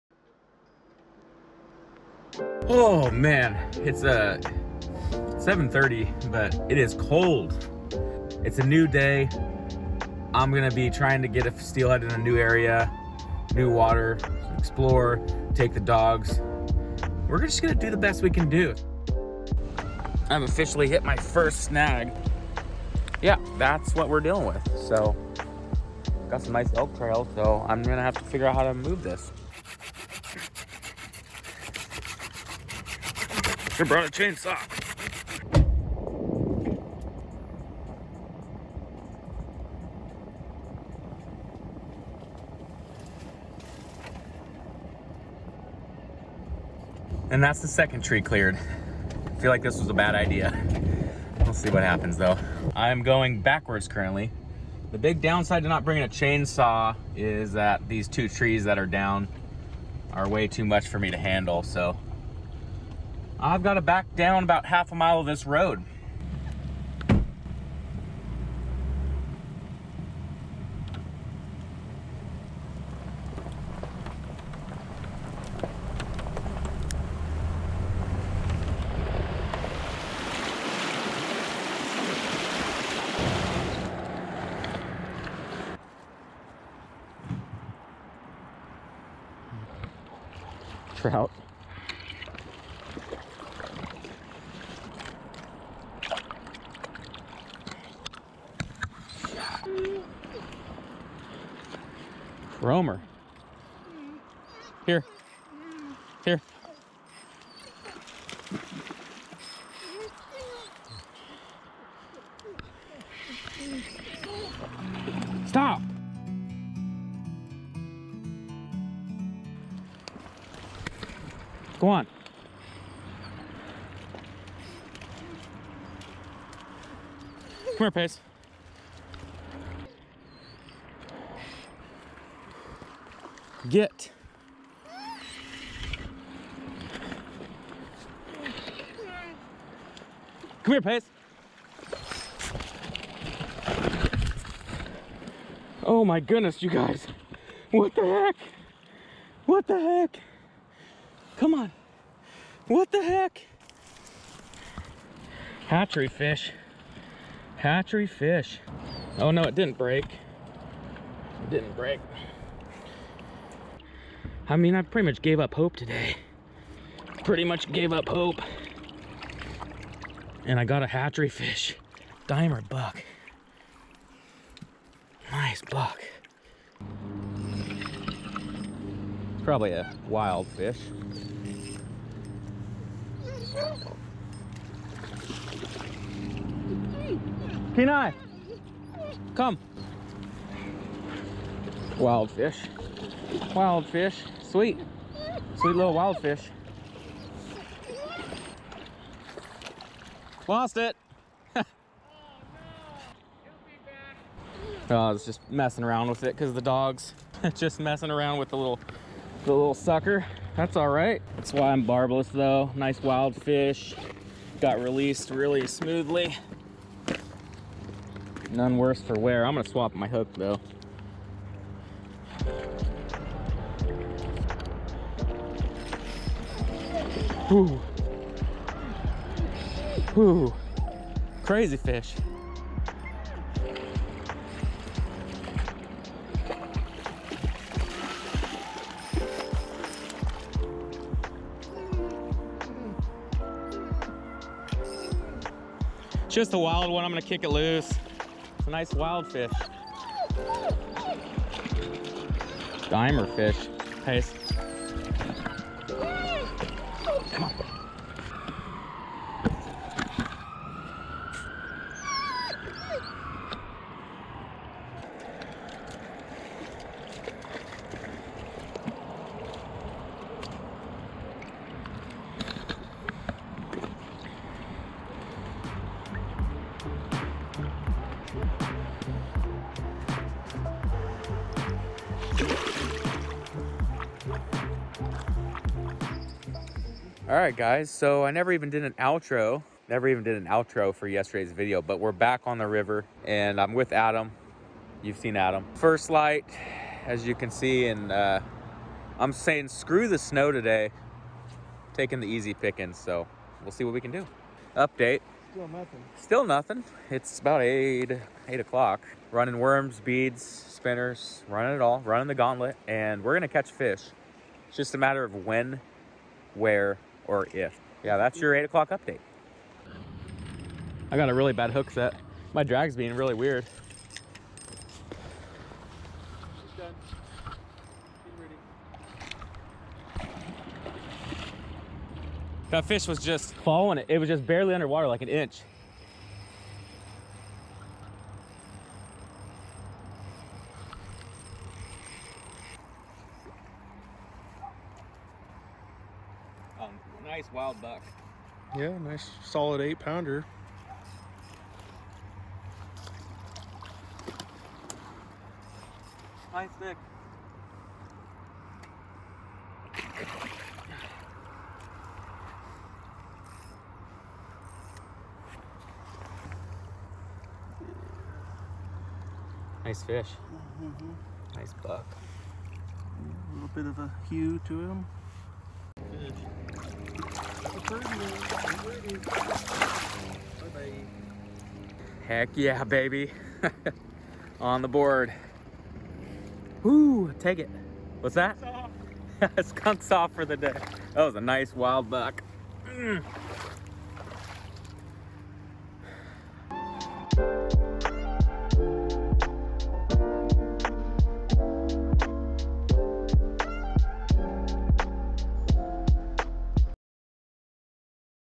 Finding NEW STEELHEAD waters! (4 fish hooked!)
Been a while since last upload, hope you enjoy this video!